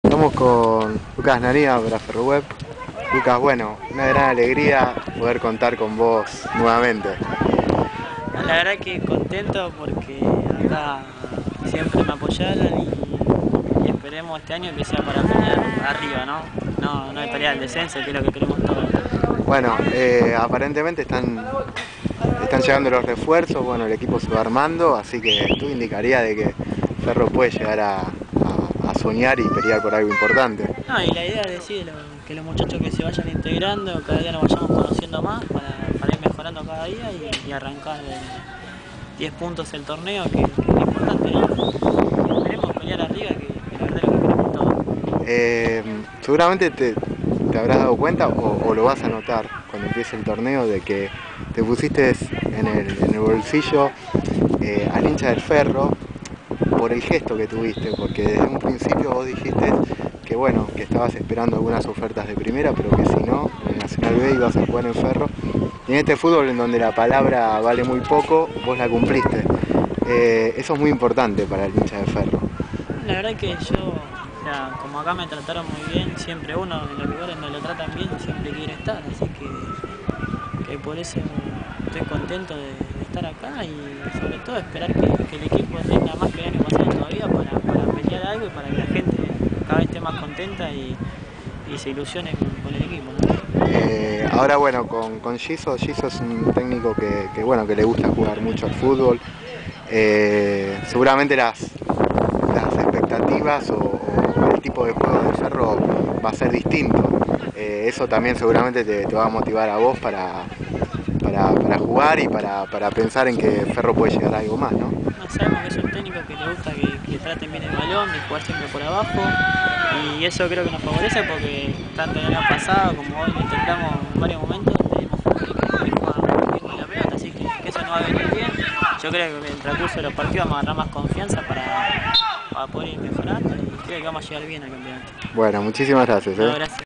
Lucas Nanía - FerroWeb
Entrevista a Lucas Nanía
Amistoso frente al CEFAR
CEFAR 1 - FERRO 2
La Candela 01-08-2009